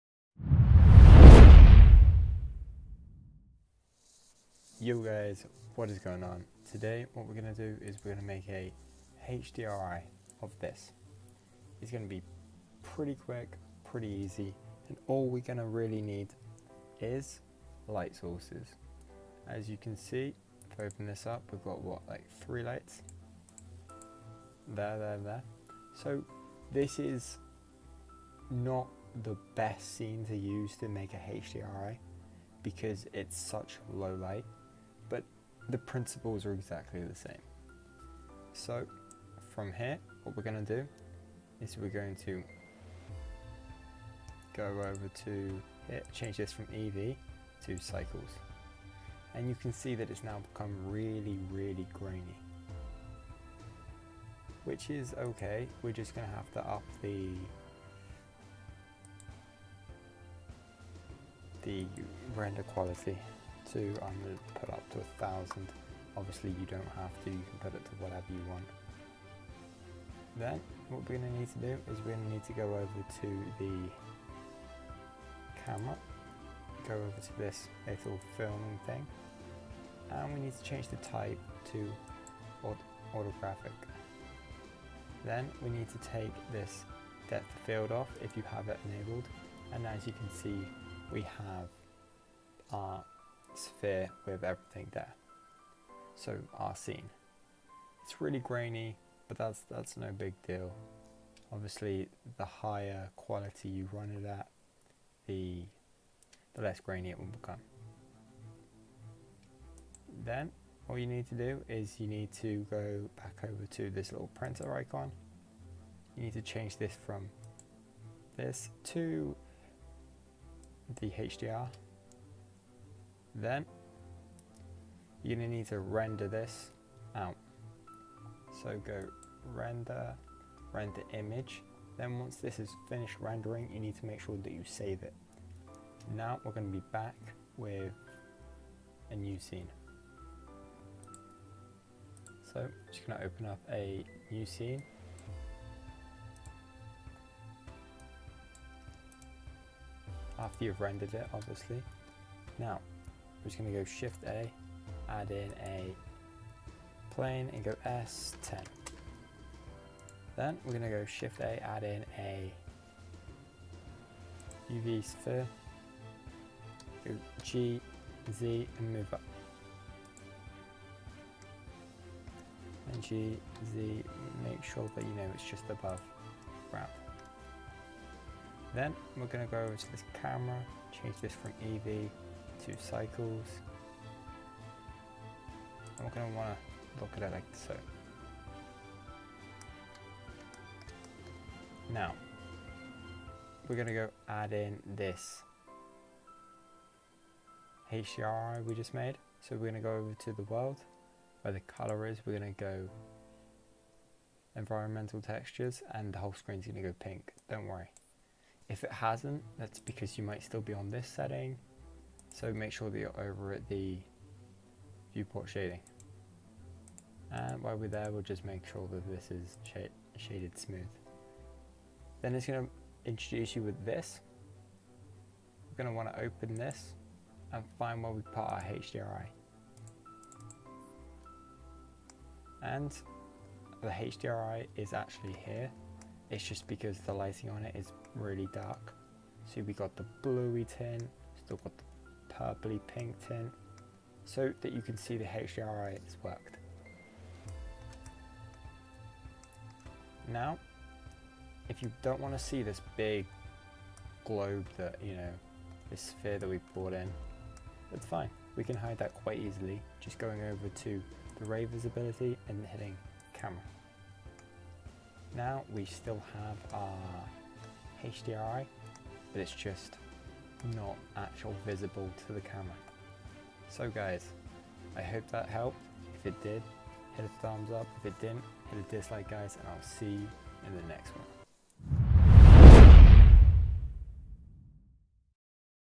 HDRI In Blender (Blender Tutorial 2.93)
In this tutorial, I will show you how make an Easy HDRI in Blender using cycles.

If you would like to make this and animate it. Here is a link to that video. Quick & Easy Animation in Blender (Blender Tutorial 2.93)

Also, if you like this, smash the like button it helps out a lot.

Jgdnetwork